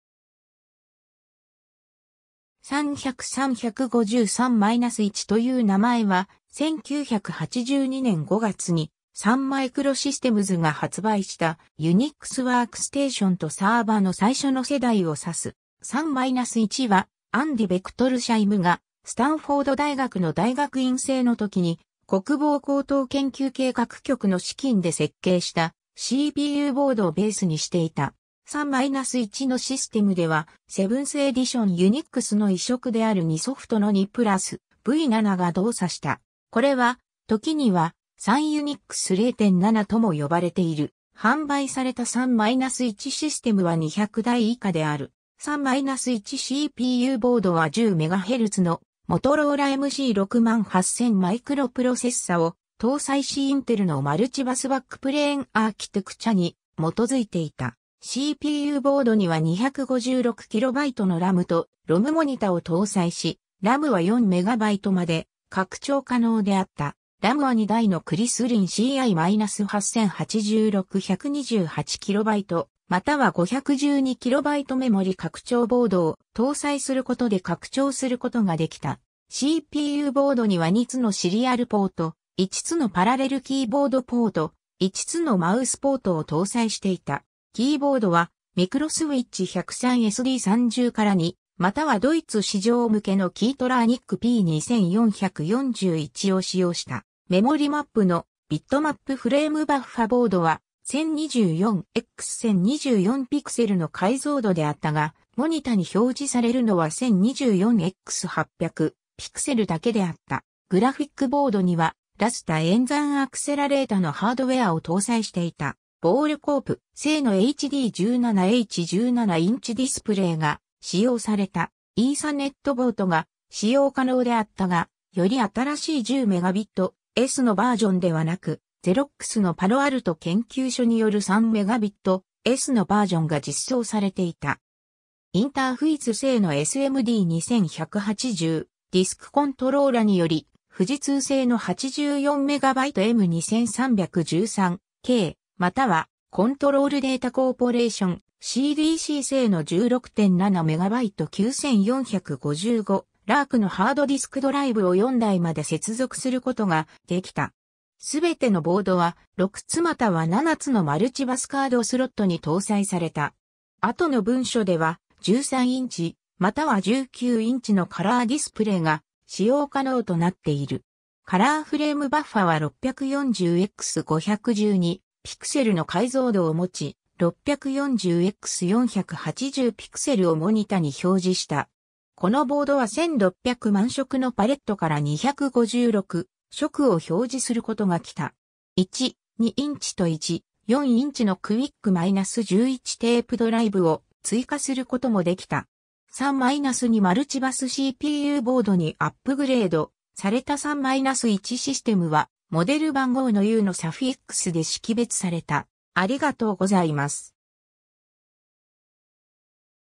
Sun-1
サン・マイクロシステムズ
Sun 100 Sun 150 Sun-1 という名前は、1982年5月にサン・マイクロシステムズが発売した、UNIXワークステーションとサーバの最初の世代を指す。
Sun-1は、アンディ・ベクトルシャイムがスタンフォード大学の大学院生のときに、国防高等研究計画局 (DARPA) の資金で設計したCPUボードをベースにしていた。
Sun-1のシステムでは、Seventh Edition Unix の移植である UniSoft の UniPlus V7 が動作した。
これは、時には Sun UNIX 0.7 とも呼ばれている。
販売された Sun-1 システムは 200 台以下である。
 Sun-1 CPU ボードは 10 MHz のモトローラ MC68000 マイクロプロセッサを搭載しインテルのマルチバスバックプレーンアーキテクチャに基づいていた。
CPU ボードには 256 KB の RAM と ROM モニタを搭載し、 RAM は 4 MB まで拡張可能であった。
RAM は 2 台の Chrislin CI-8086 128 KB または 512 KB メモリ拡張ボードを搭載することで拡張することができた。
CPU ボードには 2 つのシリアルポート、 1 つのパラレルキーボードポート、 1 つのマウスポートを搭載していた。
キーボードは Micro Switch 103SD30-2 またはドイツ市場向けの KeyTronic P2441 を使用した。
メモリマップの、ビットマップ フレームバッファ (グラフィック)ボードは、 1024x1024 ピクセルの解像度であったが、モニタに表示されるのは 1024x800 ピクセルだけであった。
グラフィックボードには、ラスタ演算アクセラレータのハードウェアを搭載していた。
Ball Corp. 製の HD17H 17 インチディスプレイが使用された。
イーサネットボートが使用可能であったが、より新しい 10 Mbit/s のバージョンではなく、ゼロックスのパロアルト研究所による 3 Mbit/s のバージョンが実装されていた。
Interphase 製の SMD 2180 ディスクコントローラにより、富士通製の 84MB M2313K またはコントロール・データ・コーポレーション （CDC)製の 16.7 MB (固定の 8.35 MB とリムーバブルの 8.35MB) 9455 Lark のハードディスクドライブを 4台まで接続することができた。全てのボードは、6 つまたは 7 つのマルチバスカードスロットに搭載された。 後の文書では、 13 インチまたは 19 インチのカラーディスプレイが使用可能となっている。カラーフレームバッファは 640x512 ピクセルの解像度を持ち、640x480 ピクセルをモニタに表示した。このボードは 1600 万色のパレットから 256 色を表示することがきた。1/2 インチ と 1/4 インチの QIC-11 テープドライブを追加することもできた。 Sun-2 マルチバス CPU ボードにアップグレードされた Sun-1 システムは、モデル番号の U のサフィックスで識別された。